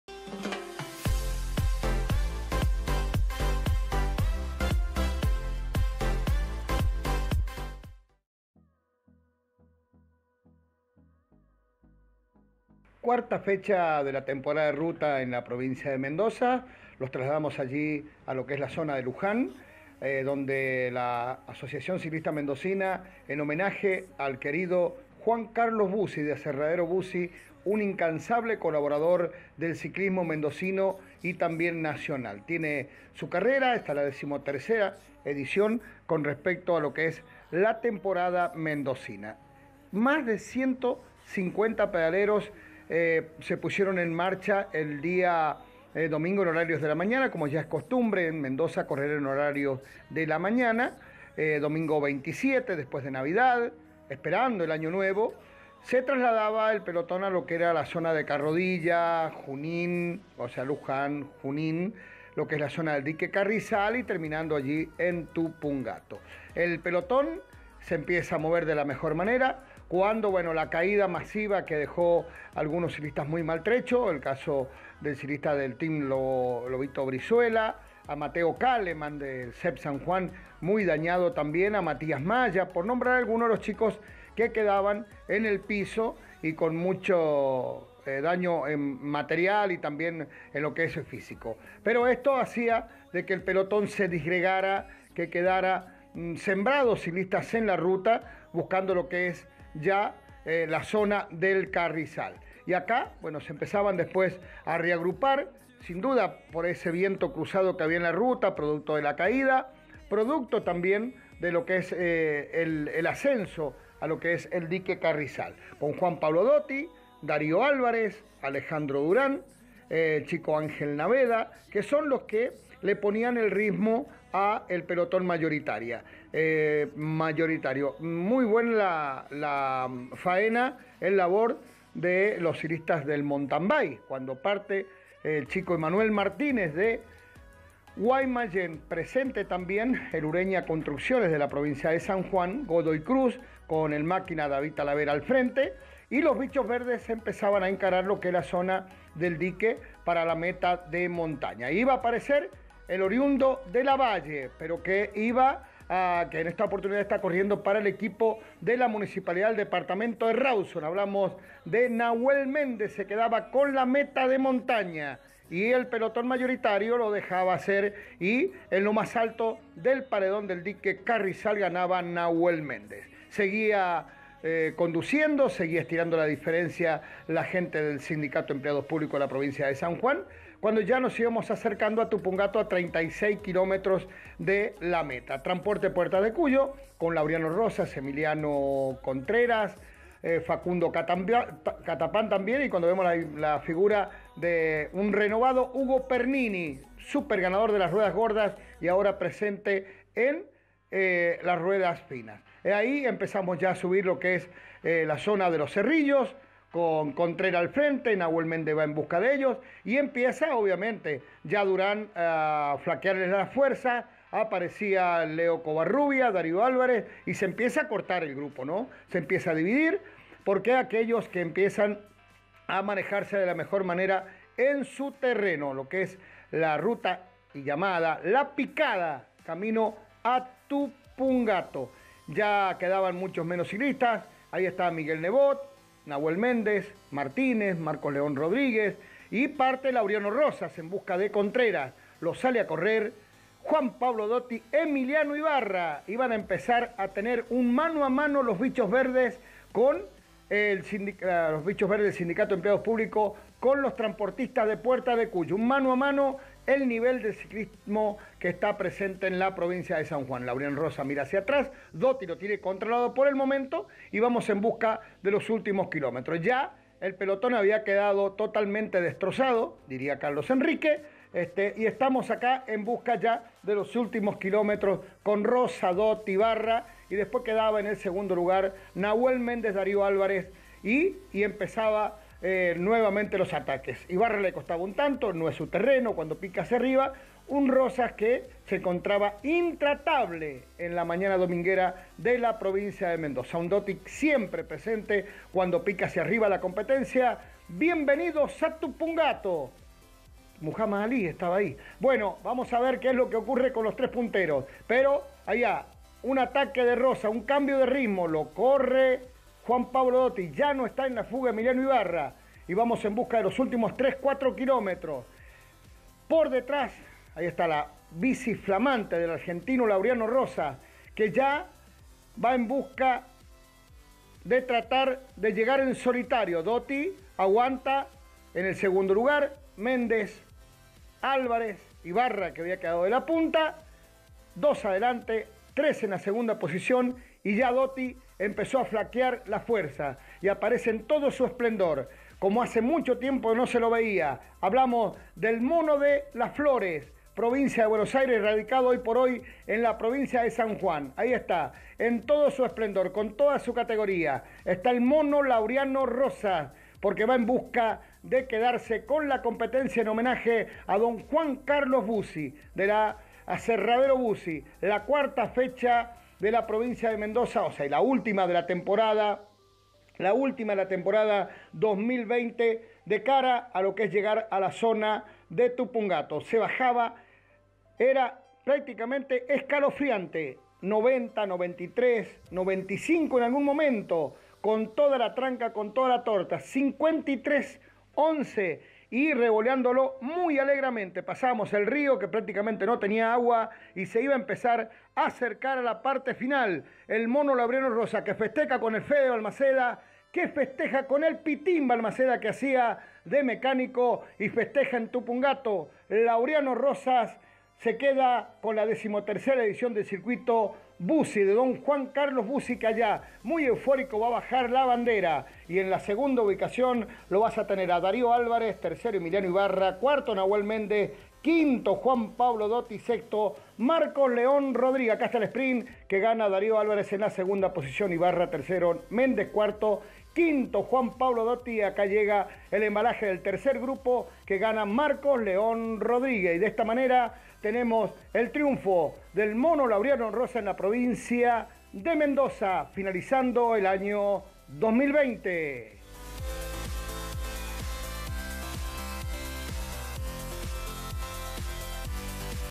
13° GP Aserradero Bucci Mendoza | Ganador Laureano Rosas
Laureano Rosas cerró el 2020 con una exhibición en una de las clásicas más bonitas del ciclismo mendocino

Clasificacion:
1- Laureano Rosas (Transporte Puertas de Cuyo) 2h43’18”
2. Darío Álvarez (SEP San Juan)
3. Emiliano Ibarra (Transporte Puertas de Cuyo)
4. Nahuel Méndez (Municipalidad de Rawson)
5. Juan Pablo Dotti (SEP San Juan)

Ciclismo total san juan 2020
Ciclismo Mendoza
Ciclismo Argentino
Ciclismo 2020